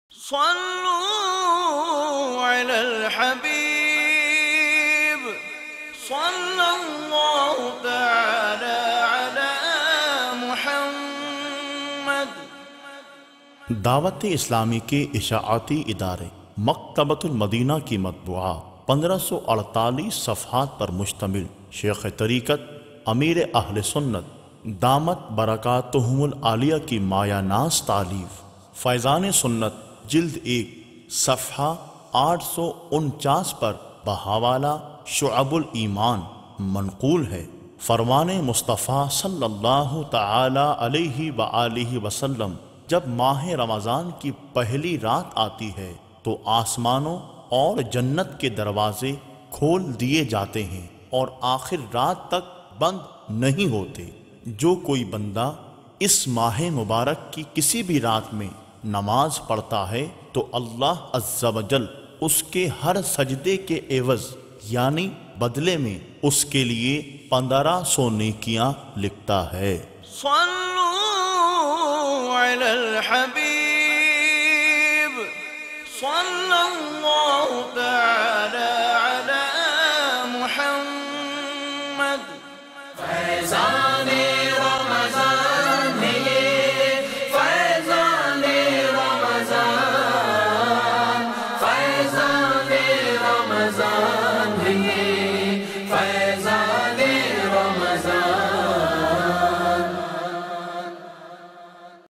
Hadees in Urdu - Mah e Ramzan ki Mubarak Ratein |HD
Mureed of Shaikh e Tareeqat Ameer e Ahle Sunnat Hazrat Allama Maulana Ilyas Qadri and Mubaligh e Dawateislami shares very good Madani Pearls in this Madani Guldasta.